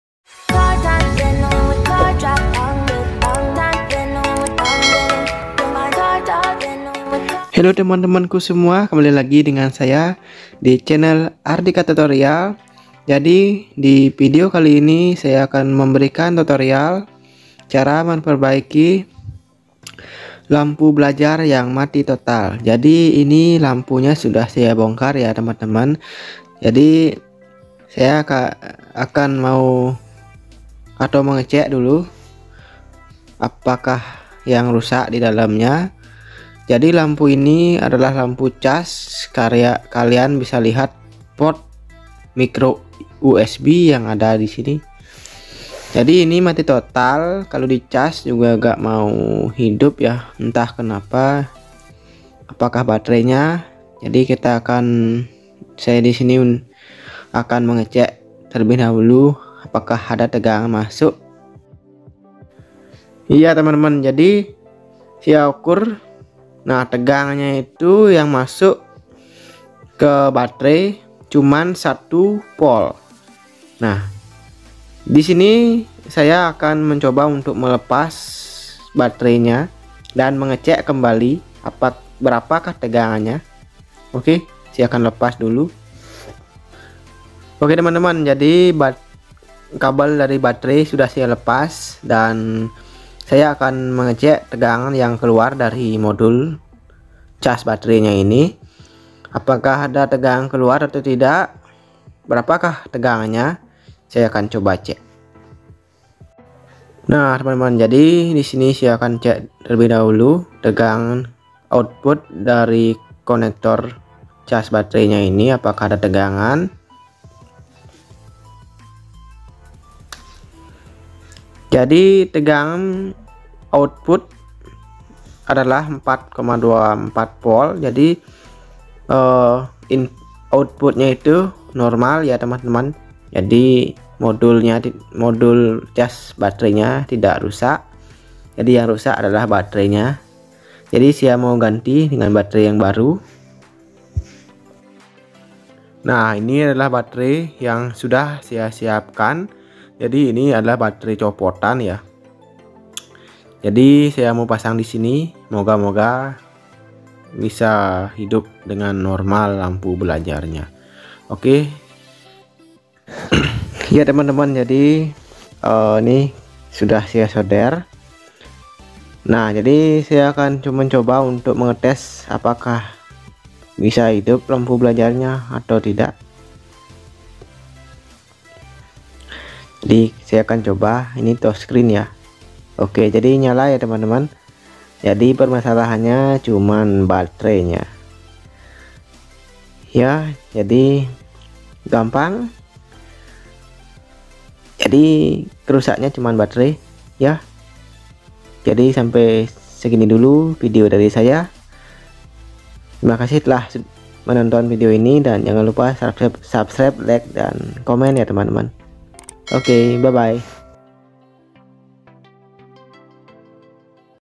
Cara memperbaiki lampu belajar yang mati
Heii temen temen
Saya bagi tips untuk kalian semua ,cara memperbaiki lampu belajar yang mati total
Dengan cara yang mudah dan tanpa harus beli baru
Dan biar kalian tidak penasaran yuk simak video nya .
Jangan di skip skip ya temen temen biar ga gagal paham hihi

3. Cara perbaiki lampy led mati total dengan sehelai kabel